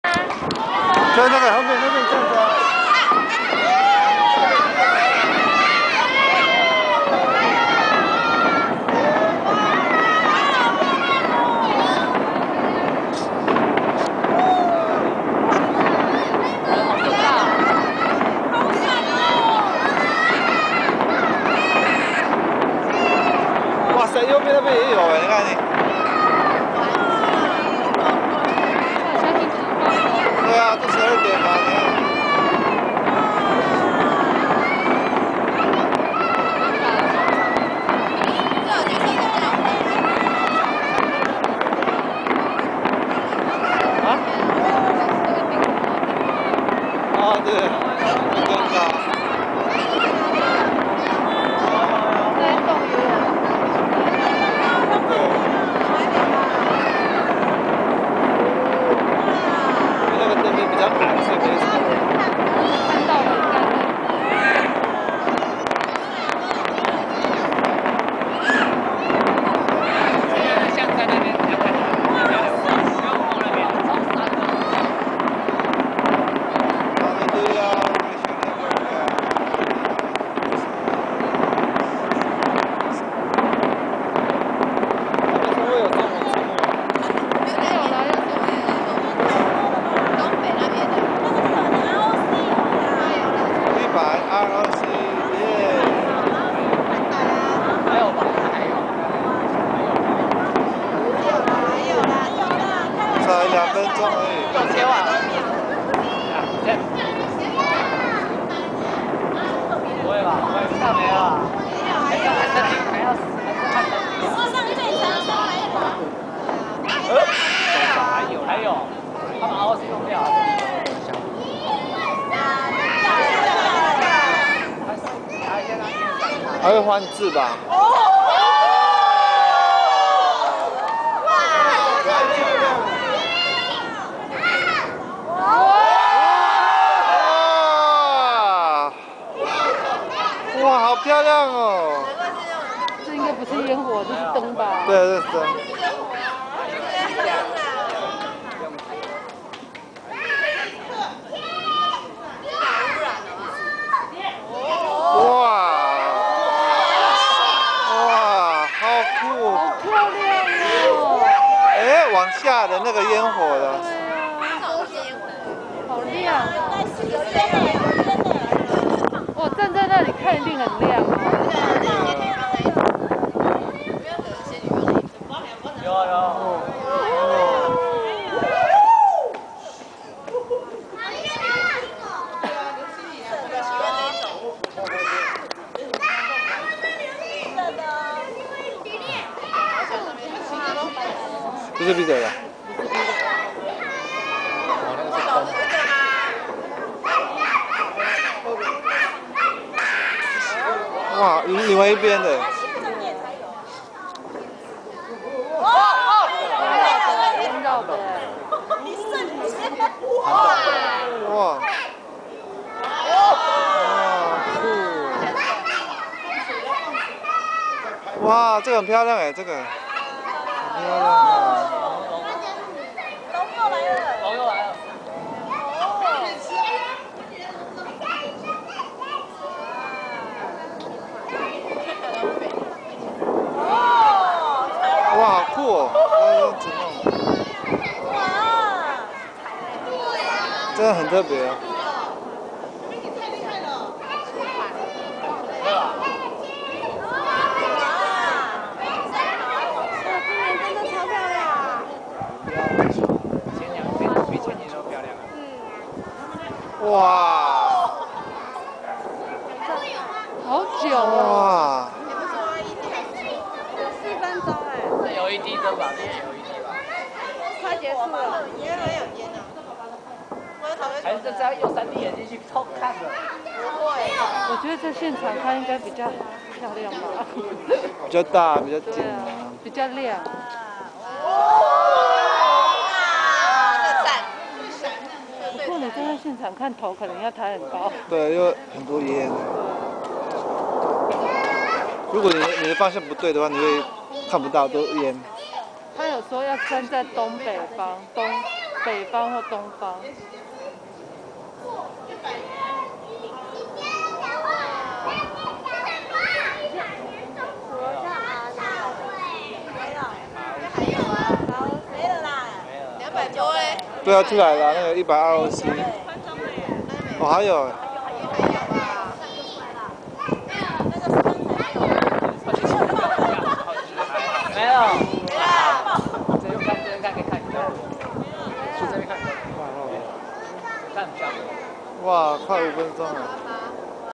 Taipei 101 New Year fireworks 2011
Taiwan's 100th year. Taipei City Fireworks for 2011.